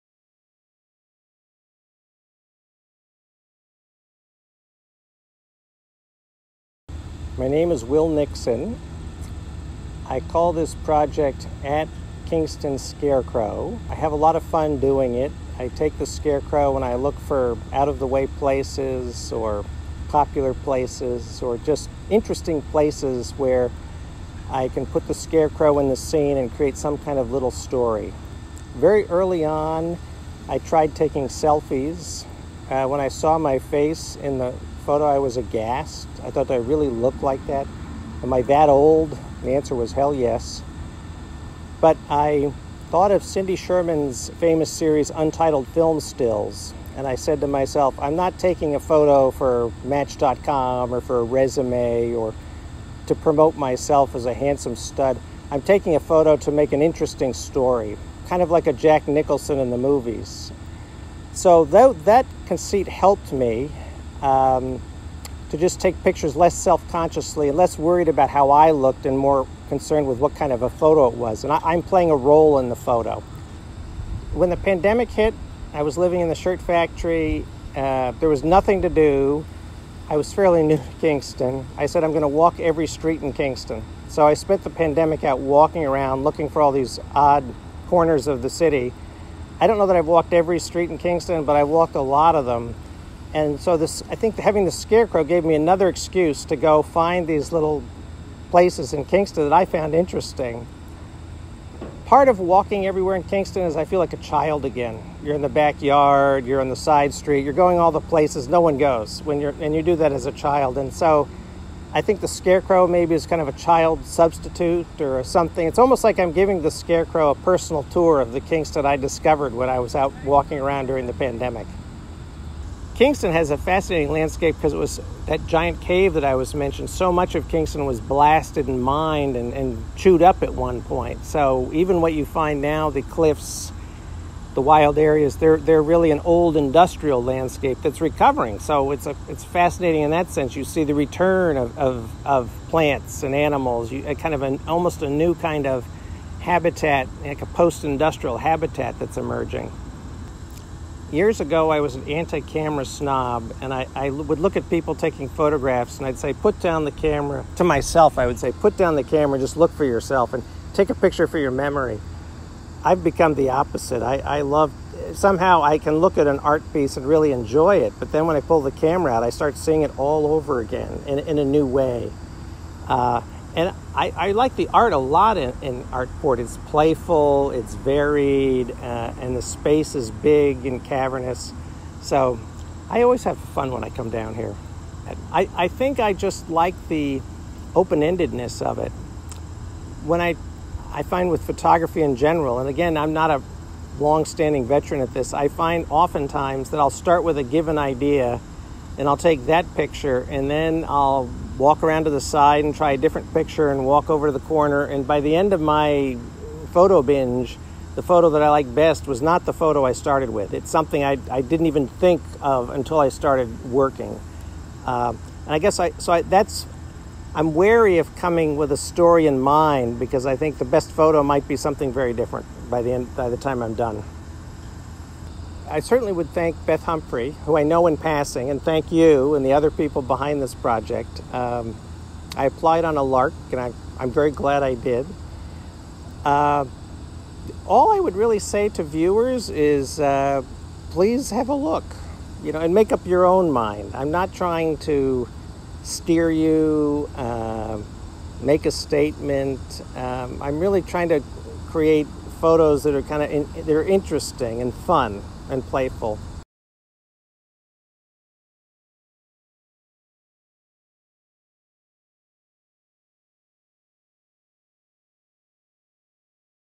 HFA2021- kingstonscarecrow by Will Nixon
Habitat For Artists @ ArtPort Kingston
kingstonscarecrow by Will Nixon

Will Nixon took a portrait of Kingston using a scarecrow as his guide and muse. See kingstonscarecrow on Instagram. Some of his favorite shots have been in the Rondout. Years ago, Nixon was an environmental journalist, more recently a several books about Woodstock and several books of poetry. In one he wrote a profile of Woodstock in poems. Now he is doing the same for Saugerties.

Follow HFA @hfaeast
HFA 2021 contact committee: Michael Asbill & Beth Humphrey
Film Production: Maxine Leu